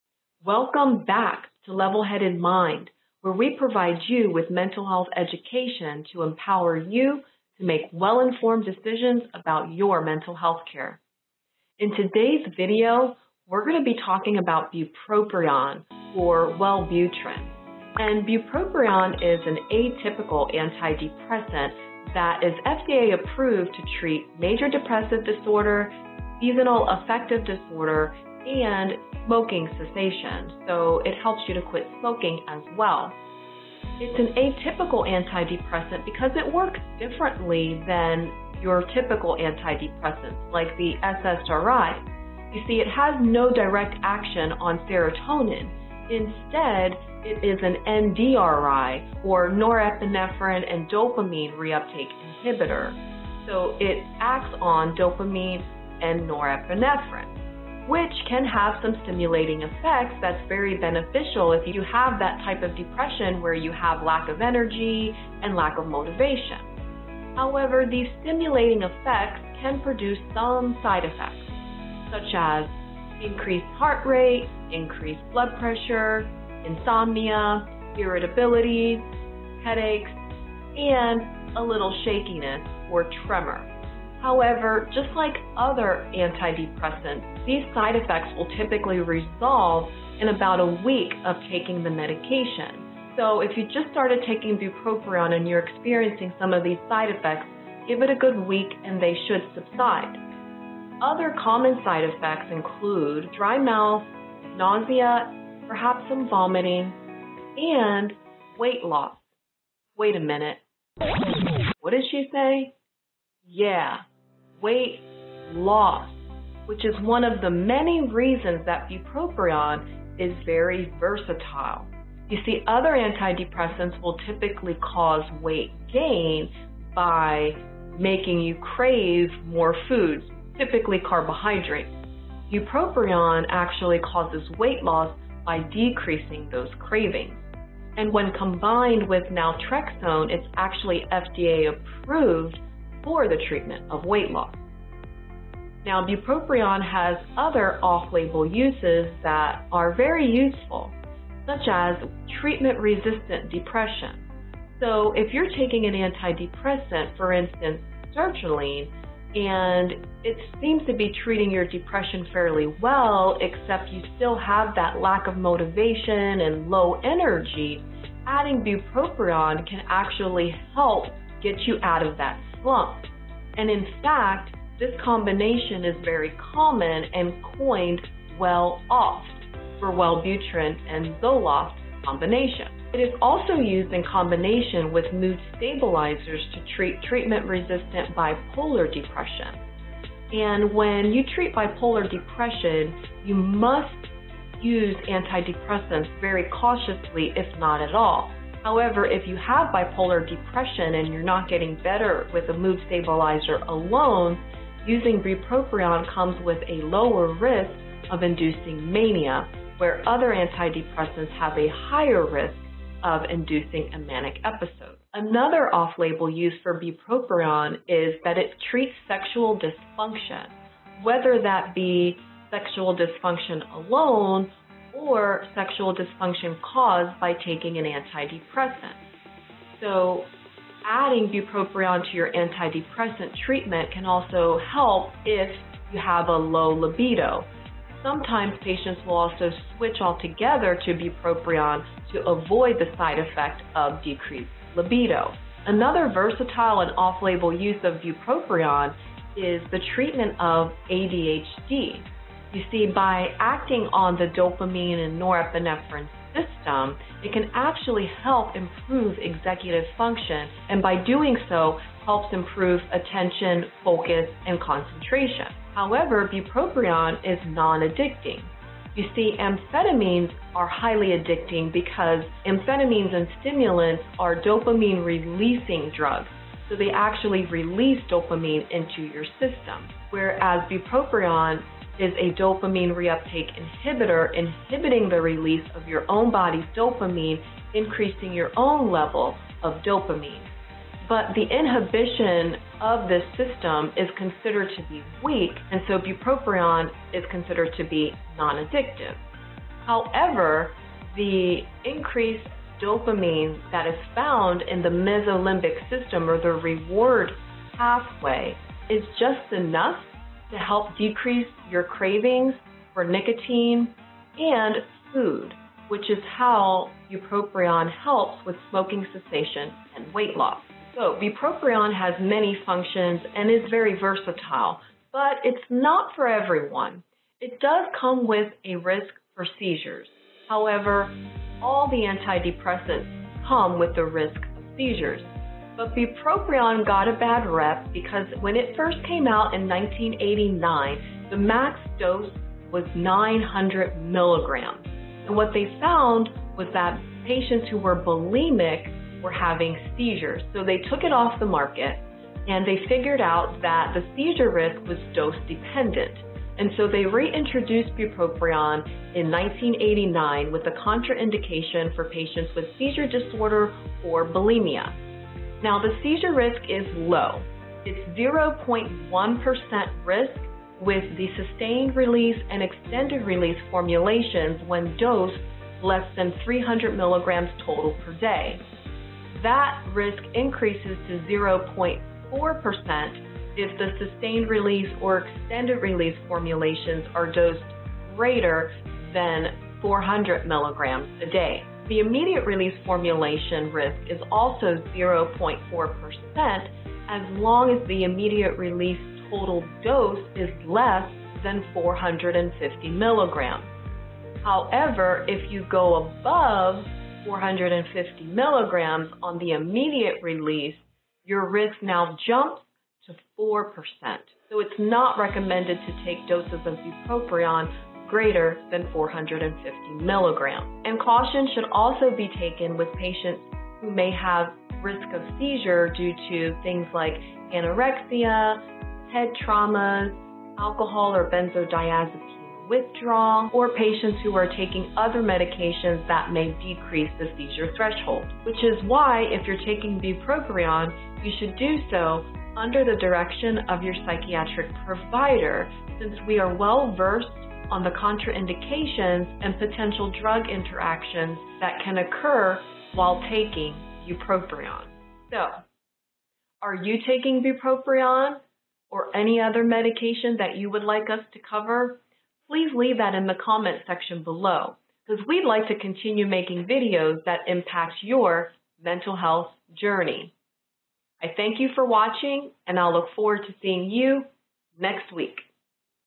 Bupropion (Wellbutrin) | The most versatile Antidepressant!
Bupropion is considered an atypical antidepressant. In today's video, we uncover the many ways Bupropion is used to not only treat depression but also used as smoking cessation treatment, weight loss, sexual dysfunction, and adjunctively with other antidepressants and mood stabilizers.

🚑 If you are in a mental health crisis, please dial 9-8-8 from the U.S. Or text HOME to 741741 and connect to a crisis counselor.

Intro 0:00
Bupropion uses 0:15
Bupropion how it works 0:35
Side effects 1:10
Off-label uses 1:55
Non-addicting 4:25
Seizure risk 5:30
Cautions 6:56
Outro 7:38

Resources:
Deprescribing Guide
Stahl's Prescribing Guide

References:
Carlat, D. & Puzantian, T. (2020). Medication Fact Book for Psychiatric Practice, 5th ed.

Costa, R., Oliveira, N. G., & Dinis-Oliveira, R. J. (2019). Pharmacokinetic and pharmacodynamic of bupropion: integrative overview of relevant clinical and forensic aspects. Drug Metabolism Reviews, 51(3), 293–313.

Finkelstein, Y., Macdonald, E. M., Li, P., Mamdani, M. M., Gomes, T., & Juurlink, D. N. (2018). Second-generation anti-depressants and risk of new-onset seizures in the elderly. Clinical Toxicology (15563650), 56(12), 1179–1184.

Stahl, S. (2017). Stahl’s Essential Psychopharmacology: Prescriber’s Guide, 6th Ed.

Stahl, S. (2021). Stahl’s Essential Psychopharmacology: Neuroscientific Basis and Practical Applications, 5th Ed.

Verbeeck, W., Bekkering, G. E., Van den Noortgate, W., & Kramers, C. (2017). Bupropion for attention deficit hyperactivity disorder (ADHD) in adults. The Cochrane database of systematic reviews, 10(10), CD009504.

Doodly v2.7.4
Adobe Premiere- video editing software
Pixabay.com